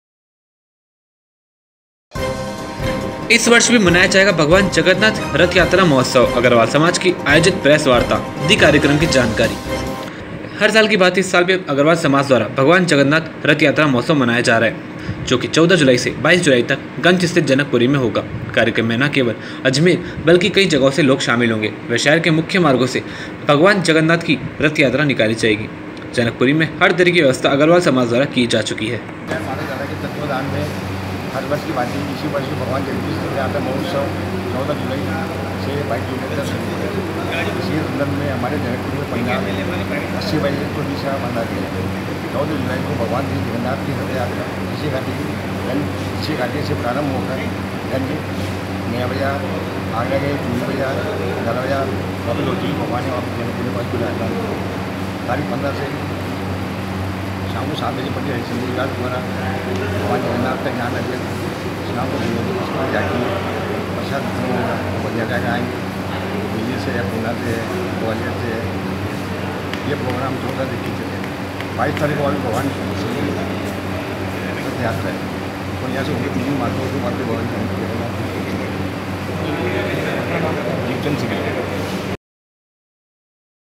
HORIZON HIND NEWS - इस वर्ष भी मनाया जाएगा भगवन जगन्नाथ रथयात्रा मोहोत्सव
इस वर्ष भी मनाया जाएगा भगवन जगन्नाथ रथयात्रा मोहोत्सव | अग्रवाल समाज की आयोजित हुई प्रेस वार्ता | दी कार्यकर्म की जानकारी |

सिटी रिपोर्ट |

हर साल की भाती इस साल भी अग्रवाल समाज द्वारा भगवन जगन्नाथ रथयात्रा मोहोत्सव  मनाया जा रहा है | जो की 14 जुलाई से 22 जुलाई तक गंज स्तिथ जनकपुरी में होगा | कार्यकर्म न केवल अजमेर बल्कि कई जगहों से लोग शामिल होंगे व् शहर के मुख्य मार्गो से भगवन जगन्नाथ की रथ यात्रा निकाली जाएगी | जनकपुरी में हर तरह की व्यवस्था अग्रवाल समाज द्वारा की जा चुकी है |